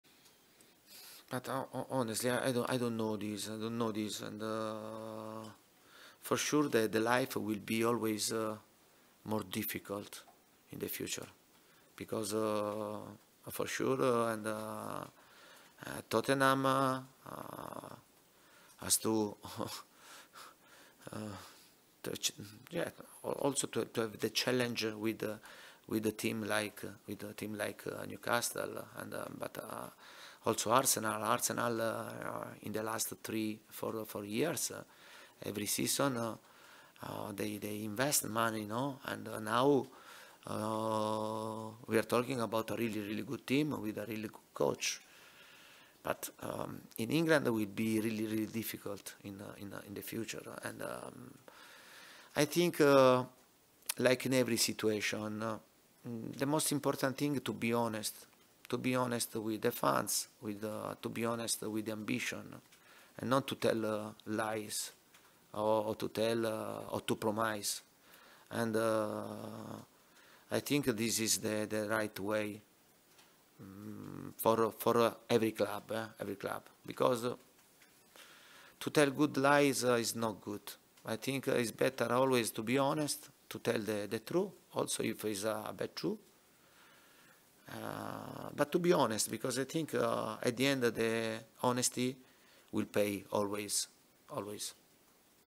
"REALLY REALLY GOOD TEAM" Tottenham Boss Antonio Conte Raves About Mikel Arteta & Arsenal | VIDEO
Tottenham Hotspur head coach Antonio Conte has raved about Mikel Arteta’s Arsenal and has praised the Gunners for the investment in the team over the years.